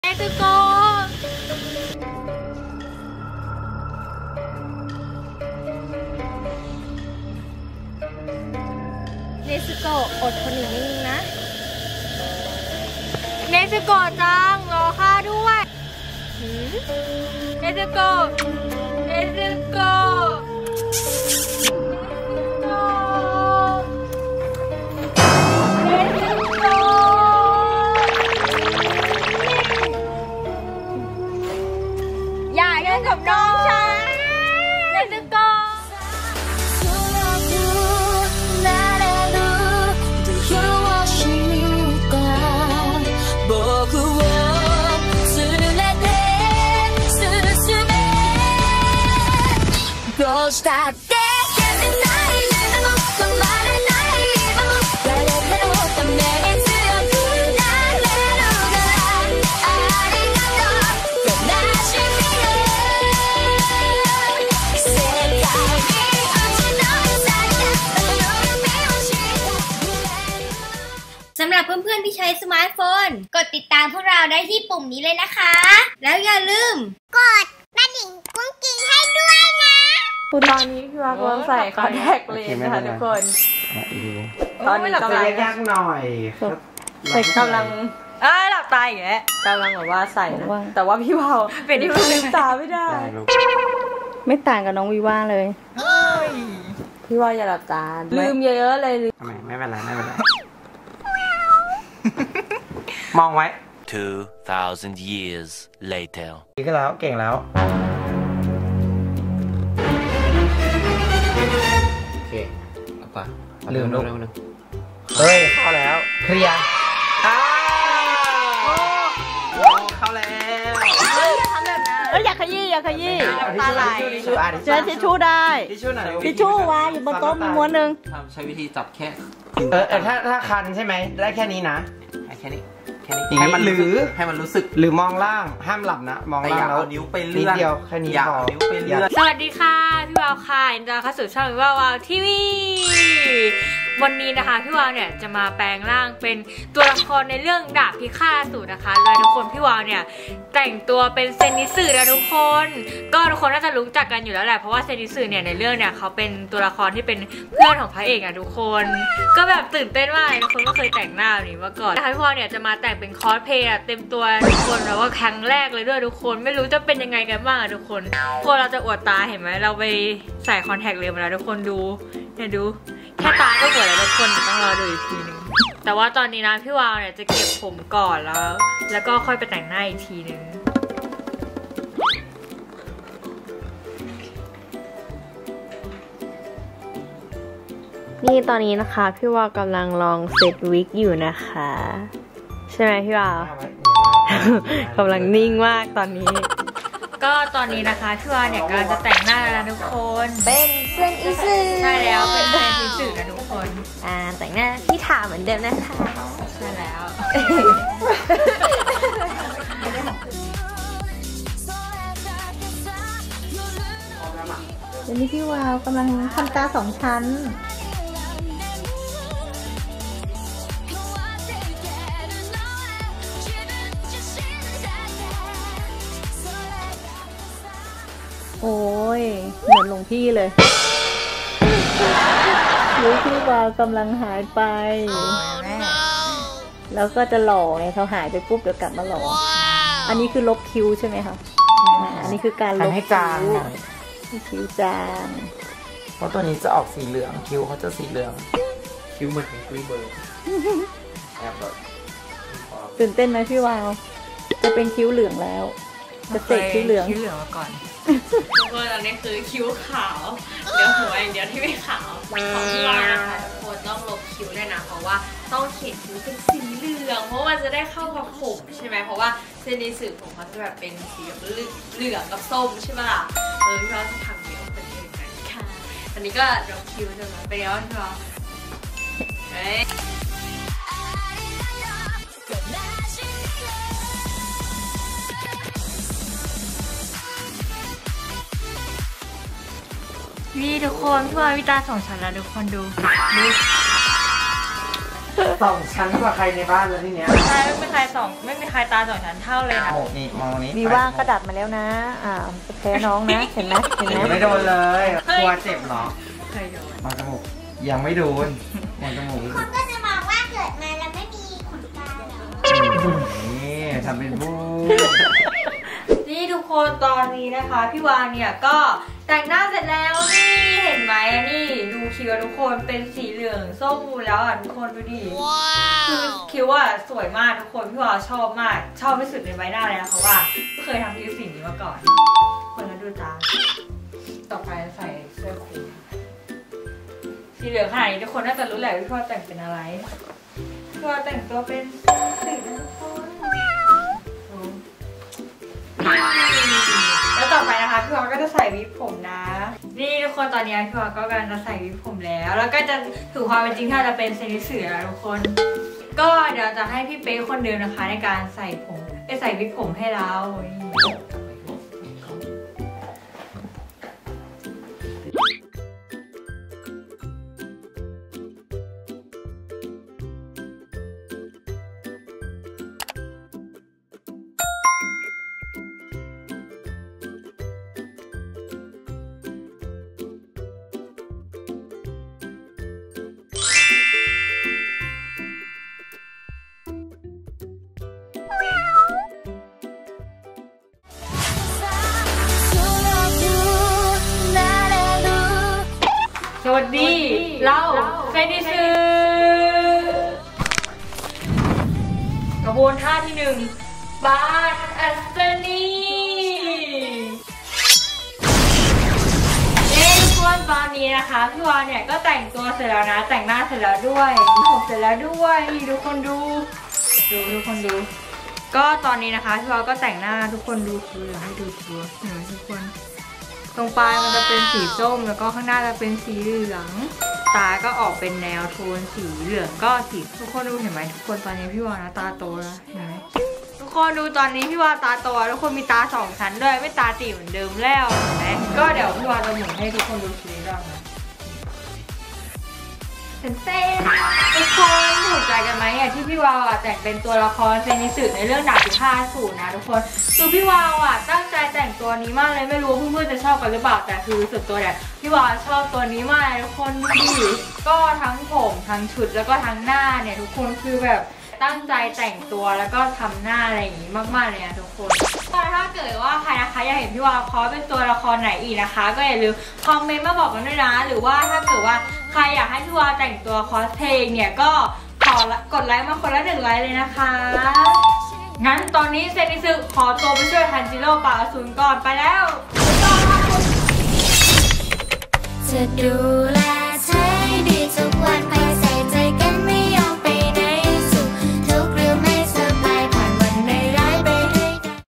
เมื่อพี่วาวกลายเป็น..เซ็นอิตสึ ดาบพิฆาตอสูร Demon slayer Kimetsu no Yaiba Cosplay | WiwaWawow TV
เมื่อพี่วาวกลายเป็น  เซ็นอิตสึ ดาบพิฆาตอสูร Zenitsu Demon slayer Kimetsu no Yaiba Cosplay | WiwaWawow TV

สติ๊กเกอร์ไลน์ My Sticker Line โหลดเลยๆ